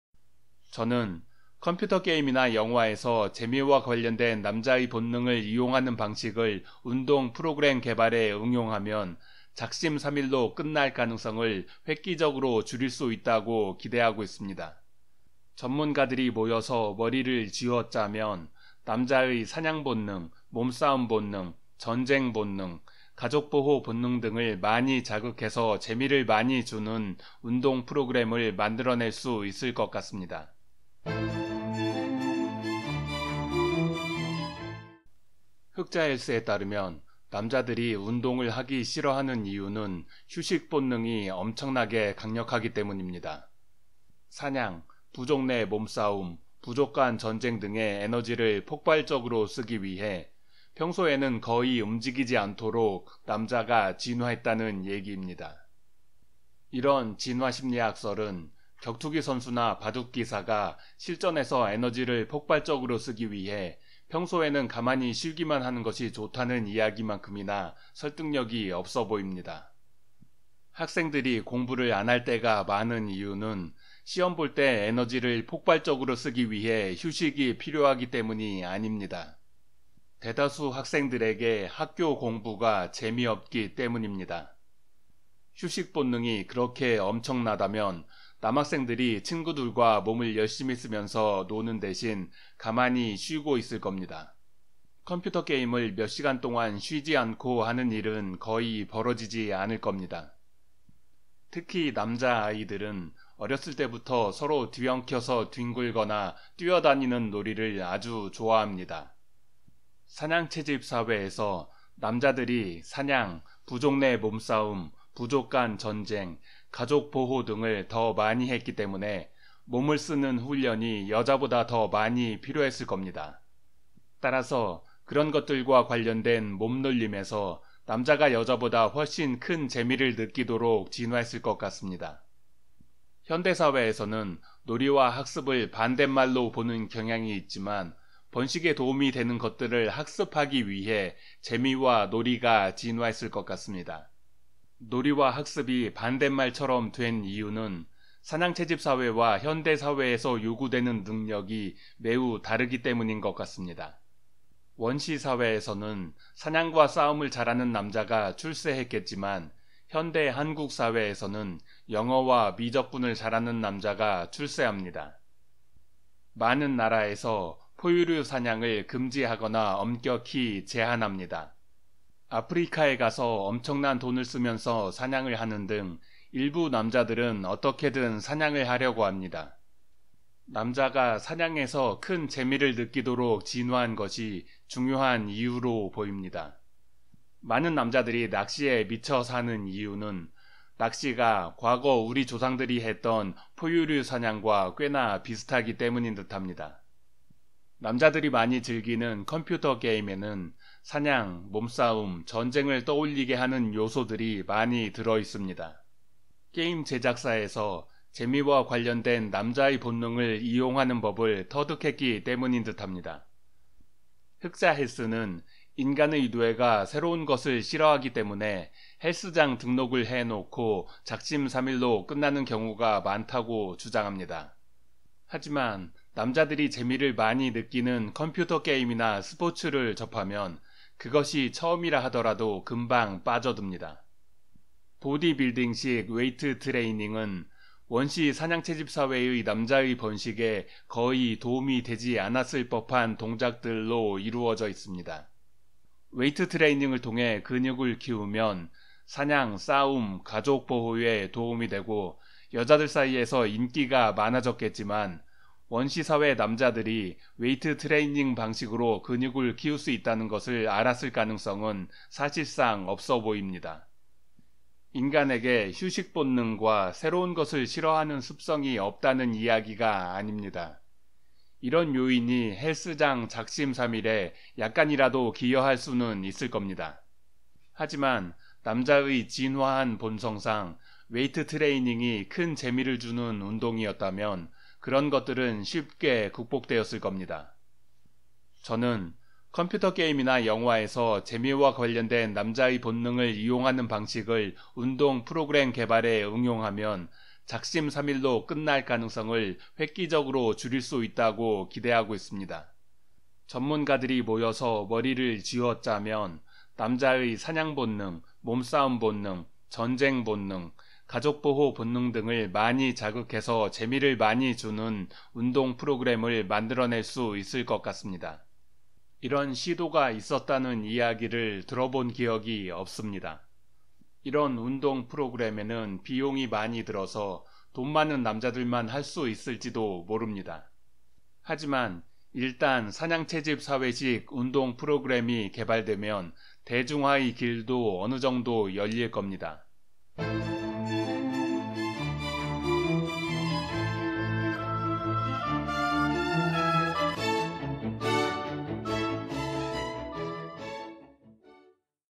엉터리 진화심리학 10. 매우 중요한 상황이 아니라면 눕고 싶도록 진화했다고? (흑자헬스 조지기 4), 재미의 진화
흑자헬스는 인간의 휴식 본능이 아주 강력해서 사냥과 전쟁 같이 아주 중요한 일이 발생하지 않으면 마냥 쉬려고 한다고 생각하는 듯합니다. 그래서 헬스장에 등록하고 작심삼일이 되기 쉽다는 겁니다.

하지만 남자들은 재미 있는 일이라면 별로 쉬지도 않고 계속하는 경향이 강합니다. 재미를 위한 낚시나 컴퓨터 게임 등이 그렇습니다. 남자는 원시 시대에 필요했던 사냥, 몸싸움, 전쟁 등과 관련된 기술을 훈련하기 위해 어렸을 때부터 놀이를 하도록 진화한 것 같습니다. 그리고 그런 것들에 재미를 많이 느끼도록 진화한 듯합니다. 그런데 보디빌딩식 웨이트 트레이닝은 그런 것들과 거리가 아주 멉니다.

「Children's risky play from an evolutionary perspective: the anti-phobic effects of thrilling experiences」, Ellen Beate Hansen Sandseter & Leif Edward Ottesen Kennair, 『Evolutionary psychology』, 2011

「Current perspectives on the biological study of play: signs of progress」, Kerrie Lewis Graham & Gordon Burghardt, 『The quarterly review of biology』, 2010

「The evolutionary psychology of war: offense and defense in the adapted mind」, Anthony C. Lopez, 『Evolutionary psychology』, 2017

「The evolution of two types of play」, Paul E. Smaldino, Elisabetta Palagi, Gordon Burghardt & Sergio M. Pellis, 『Behavioral ecology』, 2019

「The evolution of war and its cognitive foundations」, John Tooby & Leda Cosmides, 『Institute for evolutionary studies technical report』, 1988

「Fighting ability influences mate value in late adolescent men」, José Antonio Muñoz-Reyes, Ana Maria Fernandez, Luis Flores-Prado, Rómulo Guerra & Enrique Turiegano, 『Personality and individual differences』, 2015

「chapter 14. Homo virtualensis: evolutionary psychology as a tool for studying video games」, Zack Mendenhall, Gad Saad & Marcelo Vinhal Nepomuceno, 『Evolutionary psychology and information systems research』, 2010

「Hunter-gatherer studies and human evolution: a very selective review」, Kristen Hawkes, James O'Connell & Nicholas Blurton Jones, 『American journal of physical anthropology』, 2018

「Play fighting (rough-and-tumble play) in children: developmental and evolutionary perspectives」, Peter K. Smith & Jennifer M. StGeorge, 『International journal of play』, 2022

「Sex differences in sports interest and motivation: an evolutionary perspective」, Robert O. Deaner, Shea Michael Balish & Michael P. Lombardo, 『Evolutionary behavioral siences』, 2015

「Why is muscularity sexy? tests of the fitness indicator hypothesis」, David A. Frederick & Martie G. Haselton, 『Personality and social psychology bulletin』, 2007